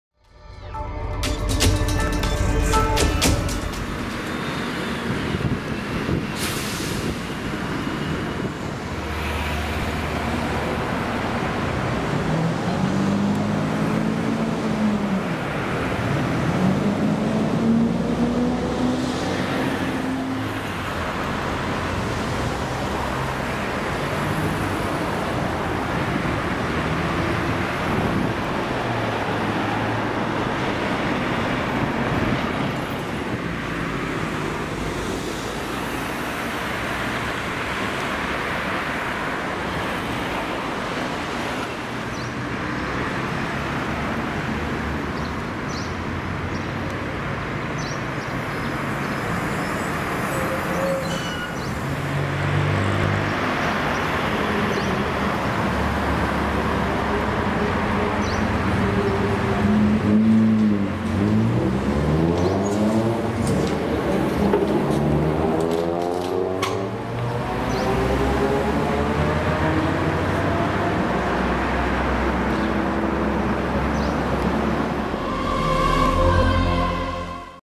Полтава 20.5.2016 Один АвтоХАМ создал неудобства
Вот так не аккуратно припарковавшись один АвтоХАМ создал неудобства очень многим проезжающим по этому маршруту автомобилям. Особо сложно приходилось водителям общественного транспорта. Из за больших габаритов им приходилось пересекать сплошную чтоб не повредить припаркованное авто.